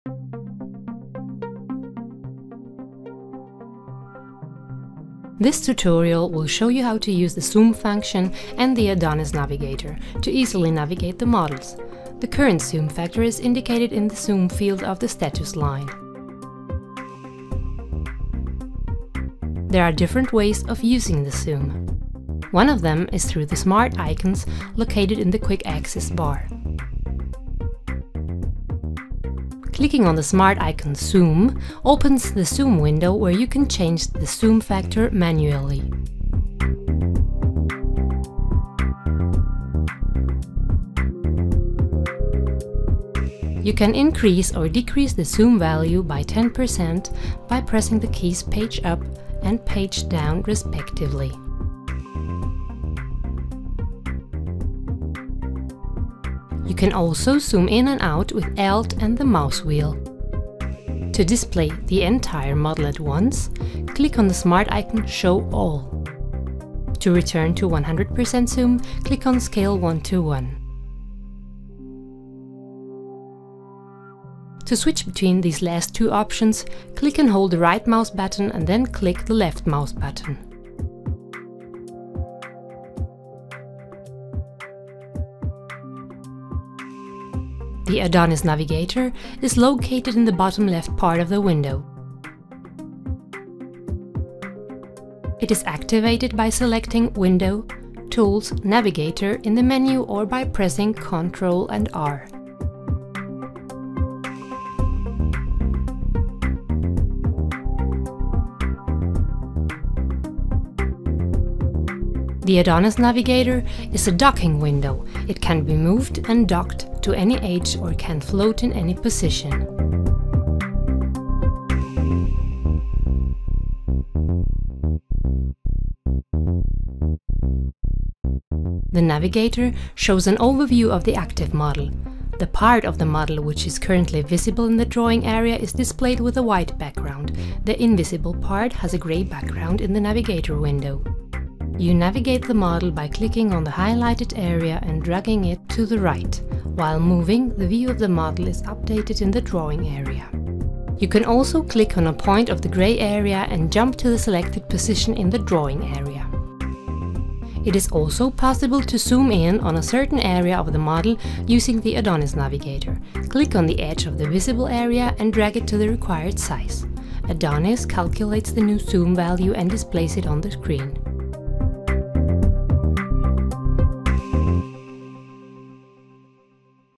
ADONIS:CE tips and tricks #3 - Zoom and Model Navigator
This video shows how to use the zoom and pan functionalities of the Model Navigator.
To learn more about free business process modelling tool ADONIS:Community Edition